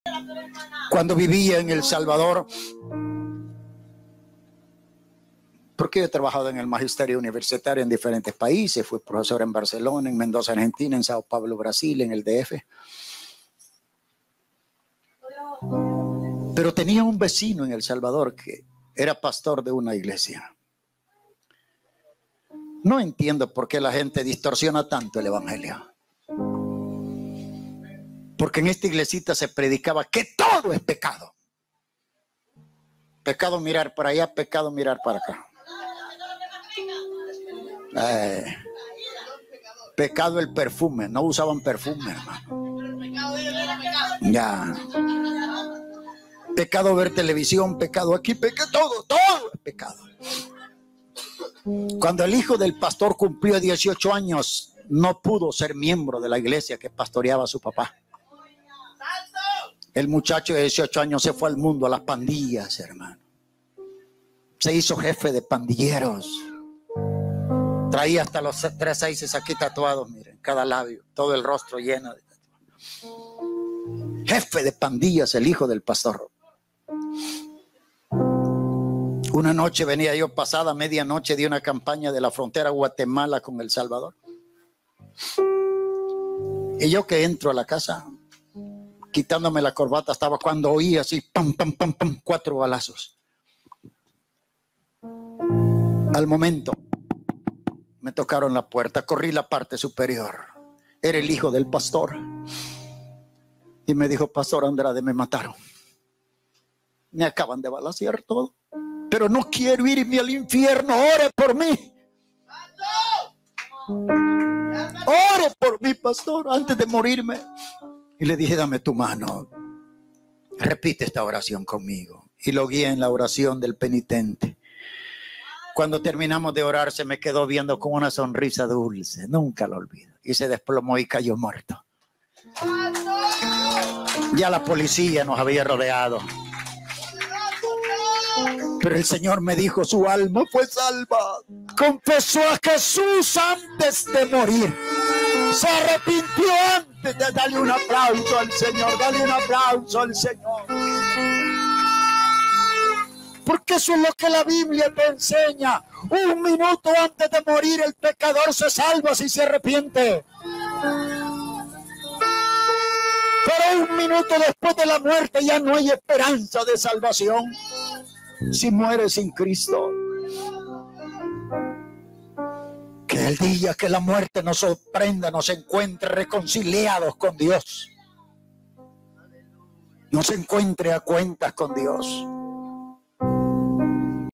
Testimonio de joven arrepentido antes de morir, aun hay esperanza - José Adán Andrade
Comparte los mensaje con tu familia y amigos.